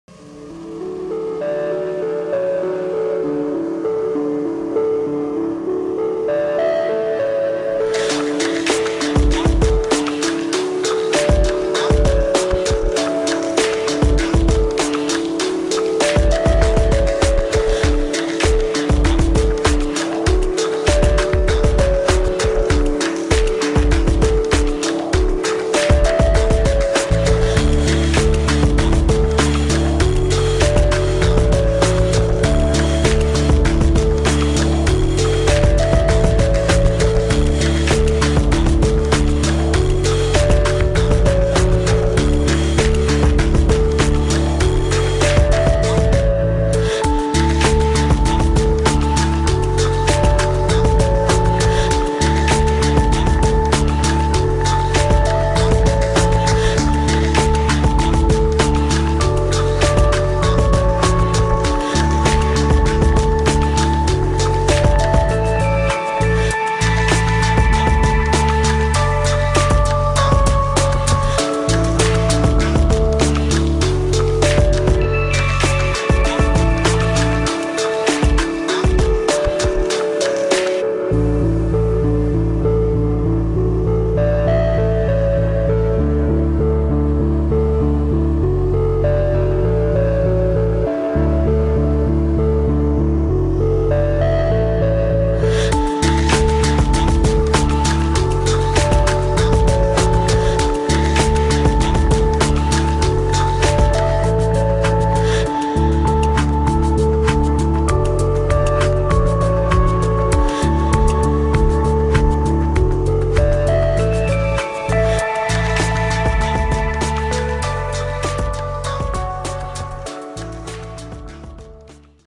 Flying over water
Little flight from a friend's boat. Nervous as hell. No flippy floppies here.